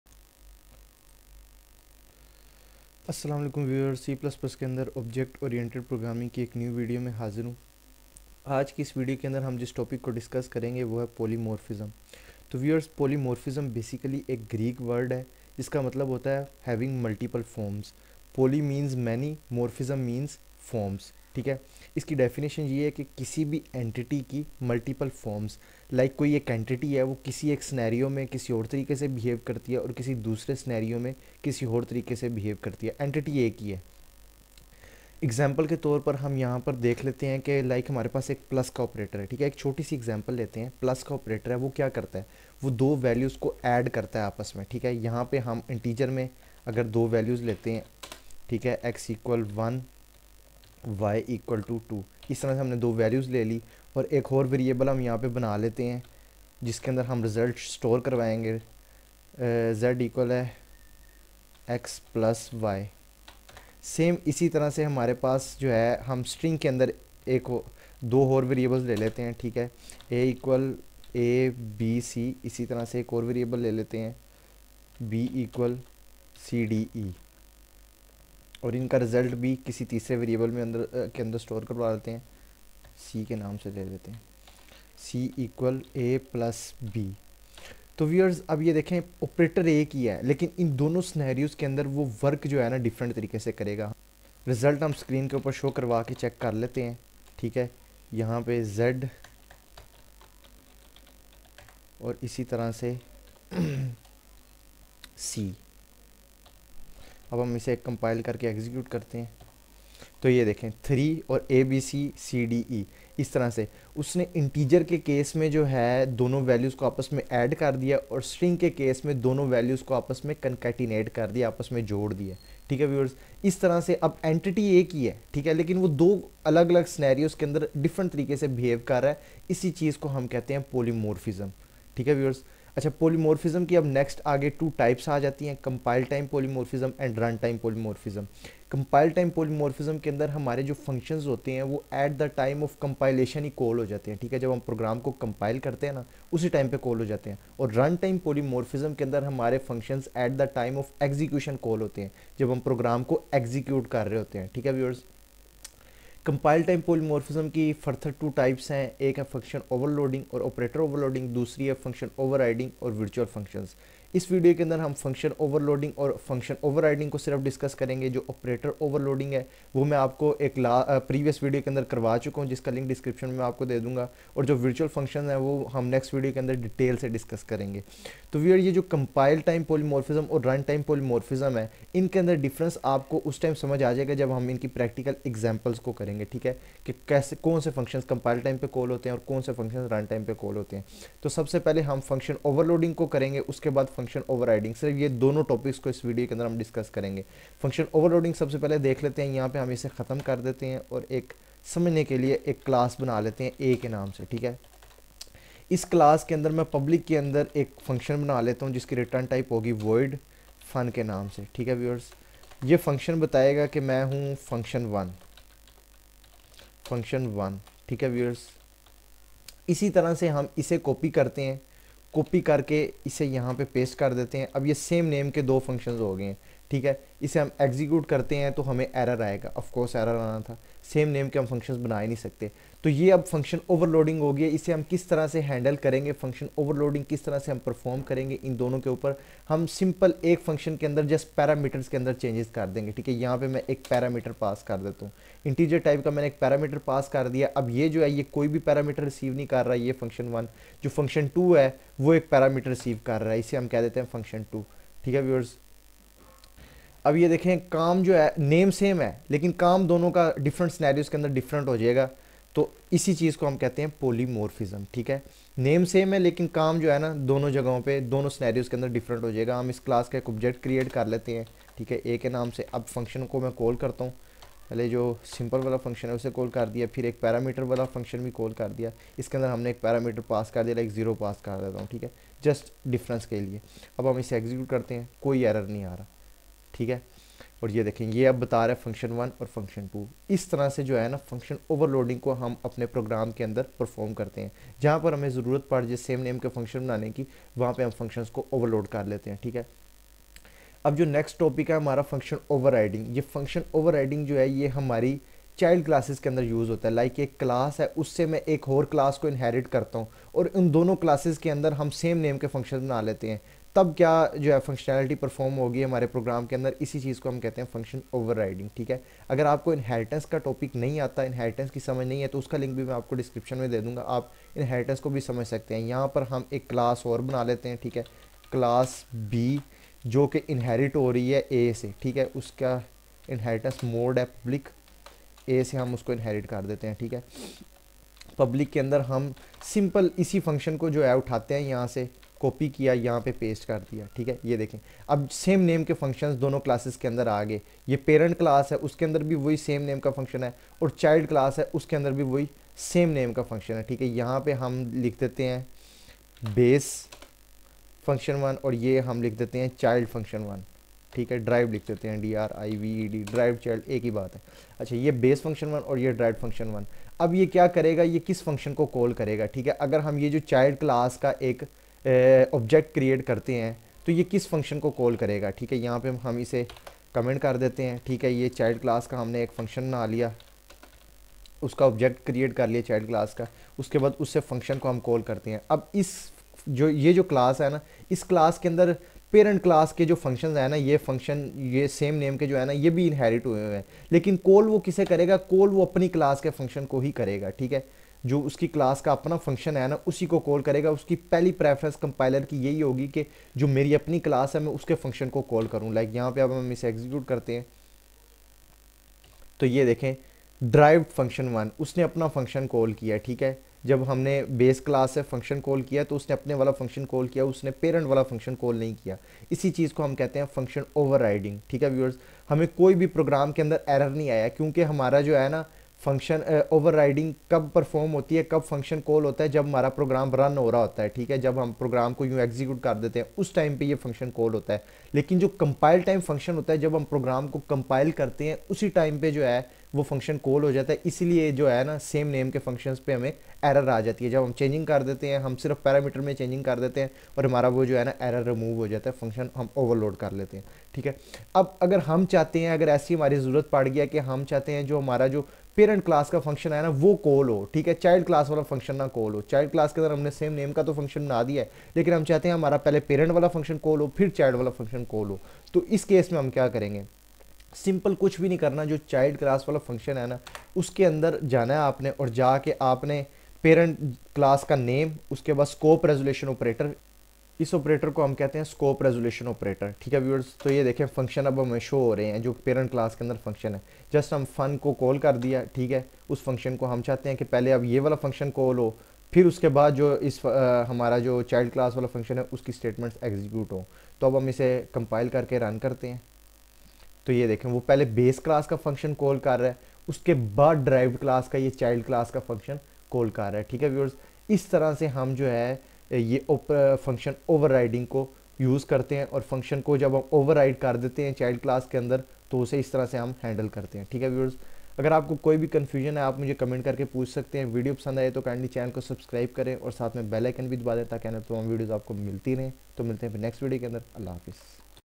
#17 Polymorphism OOP C++ in Urdu/Hindi
Polymorphism and its types
compile time polymorphism
-function overloading
-operator overloading
run time polymorphism
-function overriding
-virtual function